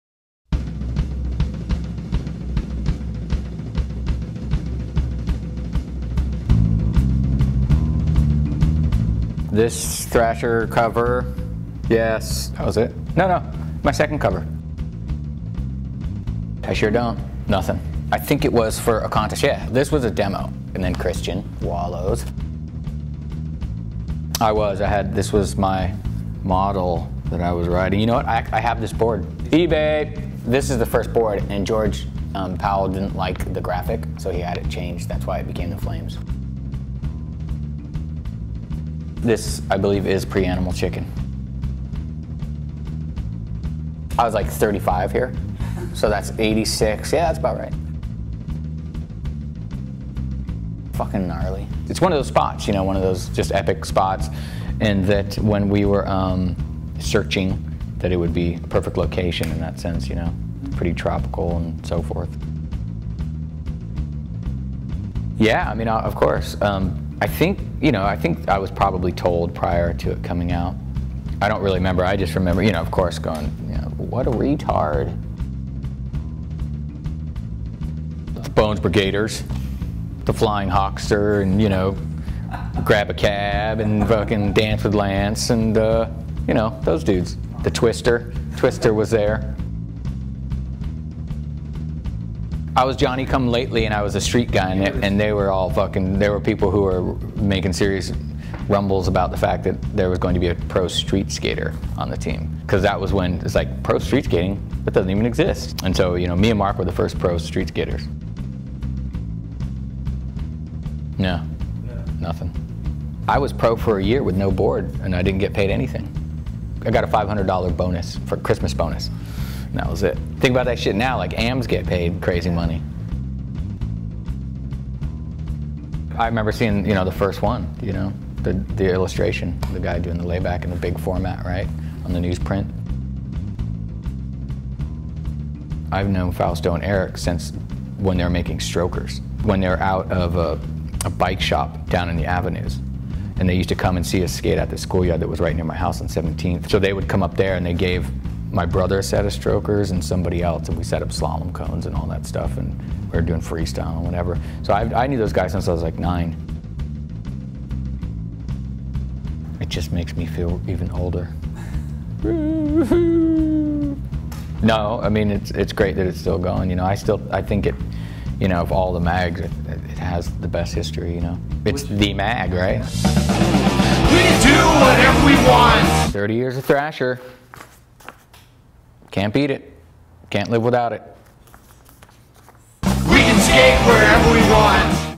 Classic Covers: Tommy Guerrero
Having been an eye-witness to and direct participant in skate history for over 30 years, Tommy has a lot to say about The Mag, Wallows, and Powell Peralta.